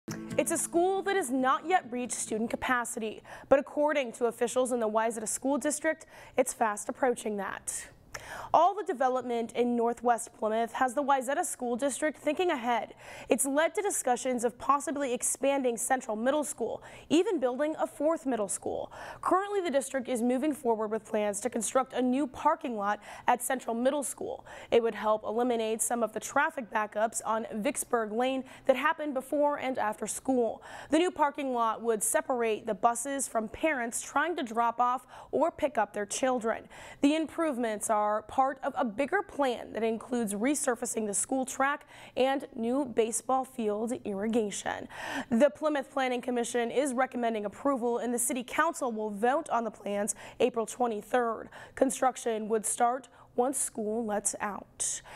Upgrades Planned for Central Middle School
All the development in northwest Plymouth has the Wayzata School District thinking ahead. It’s led to discussions of possibly expanding Central Middle School and even building a fourth middle school.

Currently, the district is moving forward with plans to construct a new parking lot at Central Middle School. It would help eliminate some of the traffic backups on Vicksburg Lane that happen before and after school.

The new parking lot would separate the buses from parents trying to drop off or pick up their children.

The improvements are part of a bigger plan that includes resurfacing the school track and new baseball field irrigation.

The Plymouth Planning Commission is recommending approval.

The city council will vote on the plans April 23. Construction would start once school lets out.